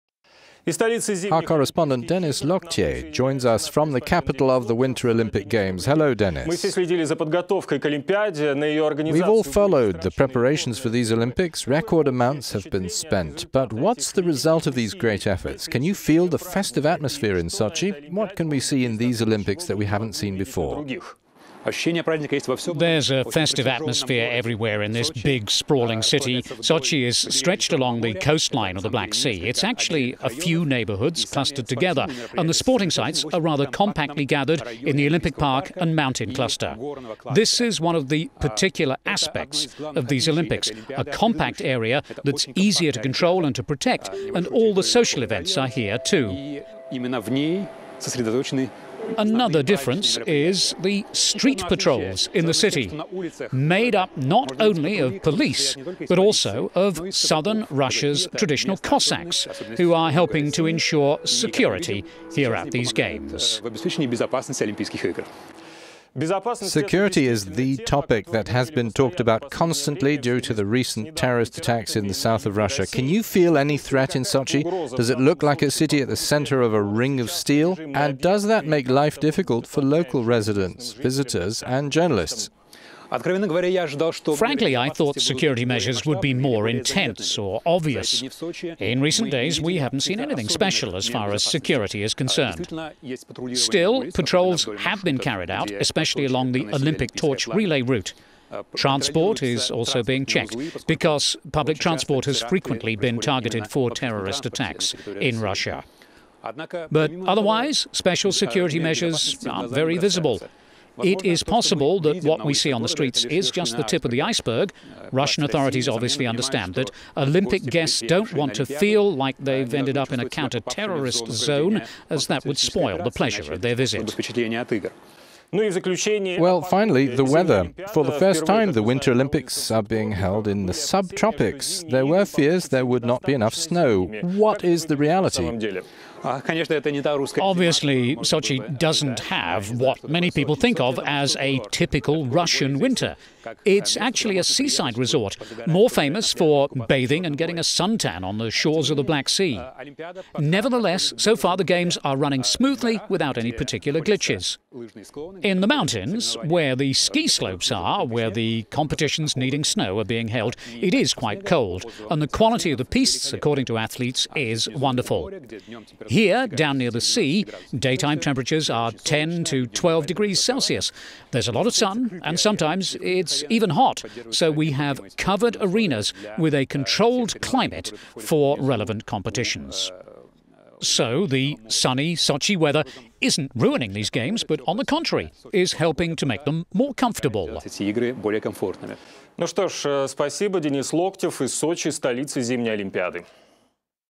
Sochi Winter Olympics: "There is a festive atmosphere everywhere"
The Winter Olympics in Sochi are now officially open.

Over the next 16 days, around 3,000 athletes will be competing for 98 medals.

It is the most expensive Olympic Games in history, with the total cost expected to be more than 37 billion euros.

Russian organisers say a record number of world leaders are attending -- triple the amount that attended the 2010 Vancouver Games.

While the build-up to the event has been overshadowed by controversy over terror threats, gay rights and unfinished hotels, organisers hope the focus can now be on the sport.

As the Games were officially declared open at a glittering ceremony, we tested the temperature on the ground with our correspondent Denis Loctier.

Andrei Belkevich, euronews:

"We have all followed the preparations for these Olympics. Record amounts have been spent. But what about the results of these great efforts? Can you feel the festive atmosphere in Sochi? What can we see in these Olympics that we haven't seen before?"

Denis Loctier, euronews correspondent:

"There is a festive atmosphere everywhere in this big, sprawling city. Sochi is stretched along the coastline of the Black Sea. It's actually a few neighbourhoods clustered together and the sporting sites are rather compactly gathered in the Olympic Park and mountain cluster.

"This is one of the particular aspects of these Olympics, a compact area that is easier to control and to protect, and all the social events are here too.

"Another difference is the street patrols in the city, made not only by police but also by southern Russia's traditional Cossacks who are helping to ensure the security of the games..."